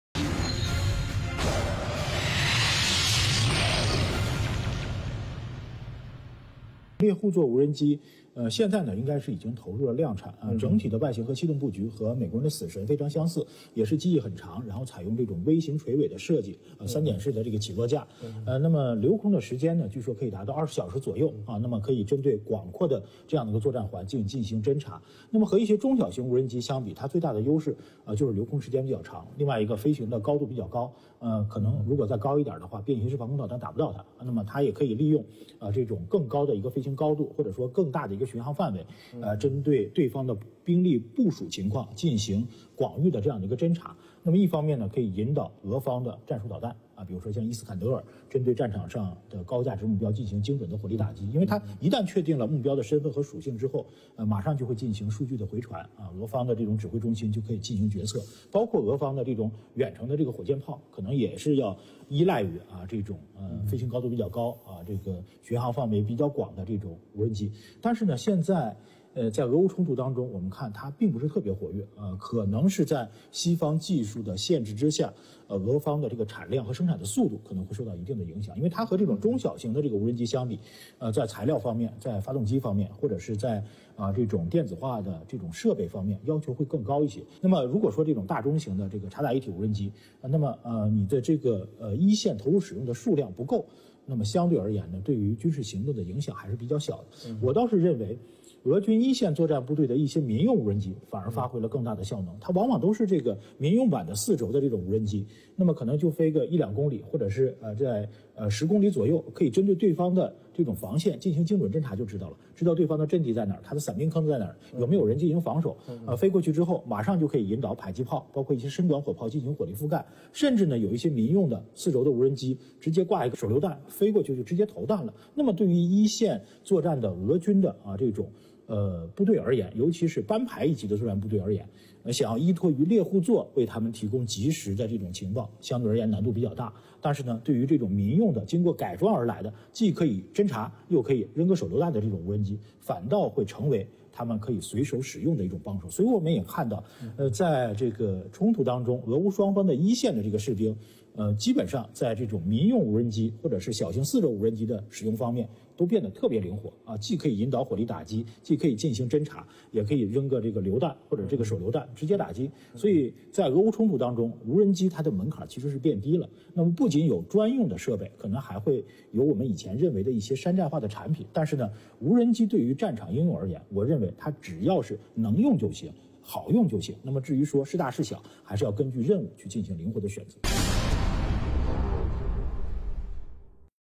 魏东旭：乌克兰军队击落过俄罗斯猎户座无人机？如何评价猎户座无人机？
详情戳视频↓揭秘。